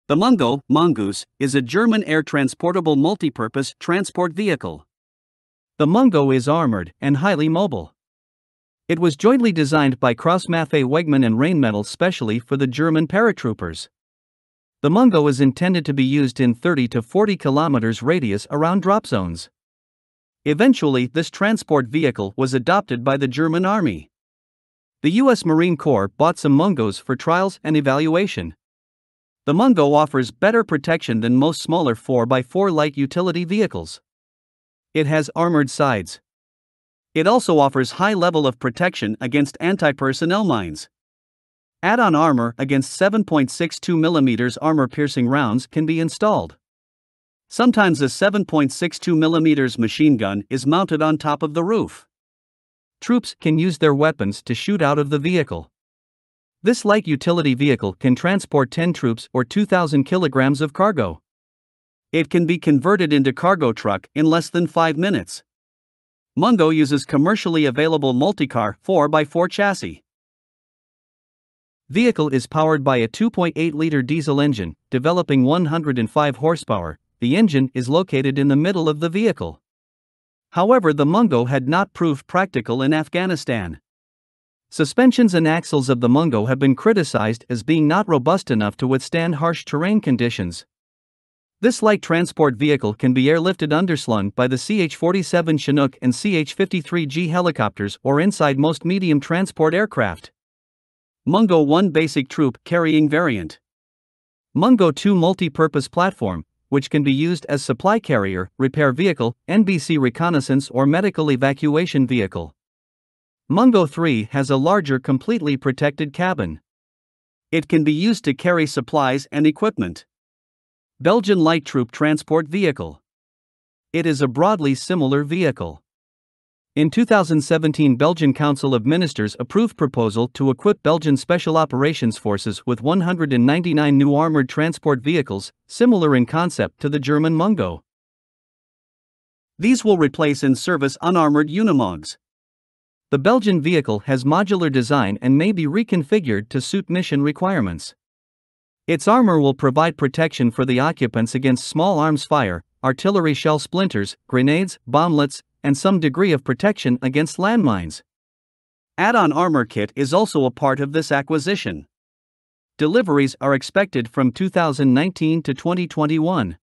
Mungo Light airborne armored vehicle
The Mungo (mongoose) is  a German air transportable multi-purpose transport vehicle. The Mungo is armored and highly mobile. It was jointly designed by Kraus-Maffei Wegmann and Rheinmetall specially for the German paratroopers. The Mungo is intended to be used in 30-40 km radius around dropzones. Eventually this transport vehicle was adopted by the German Army. The US Marine Corps bought some Mungos for trials and evaluation.

   The Mungo offers better protection than most smaller 4x4 light utility vehicles. It has armored sides. It also offers high level of protection against anti-personnel mines. Add-on armor against 7.62 mm armor-piercing rounds can be installed. Sometimes a 7.62 mm machine gun is mounted on top of the roof. Troops can use their weapons to shoot out of the vehicle.

   This light utility vehicle can transport ten troops or 2 000 kg of cargo. It can be converted into cargo truck in less than 5 minutes.

   Mungo uses commercially available Multicar 4x4 chassis. Vehicle is powered by a 2.8-liter diesel engine, developing 105 hp. the engine is located in the middle of the vehicle. However the Mungo had not proved practical in Afghanistan. Suspensions and axles of the Mungo have been criticized as being not robust enough to withstand harsh terrain conditions. This light transport vehicle can be airlifted underslung by the CH-47 Chinook and CH-53G helicopters or inside most medium transport aircraft.

Mungo 1 basic troop carrying variant.

   Mungo 2 multi-purpose platform, which can be used as supply carrier, repair vehicle, NBC reconnaissance or medical evacuation vehicle.

   Mungo 3 has a larger completely protected cabin. It can be used to carry supplies and equipment.

   Belgian Light Troop Transport Vehicle. It is a broadly similar vehicle. In 2017 Belgian Council of Ministers approved proposal to equip Belgian special operations forces with 199 new armored transport vehicles, similar in concept to the German Mungo. These will replace in service unarmored Unimogs. The Belgian vehicle has modular design and may be reconfigured to suit mission requirements. Its armor will provide protection for the occupants against small arms fire, artillery shell splinters, grenades, bomblets, and some degree of protection against land mines. Add-on armor kit is also a part of this acquisition. Deliveries are expected from 2019 to 2021.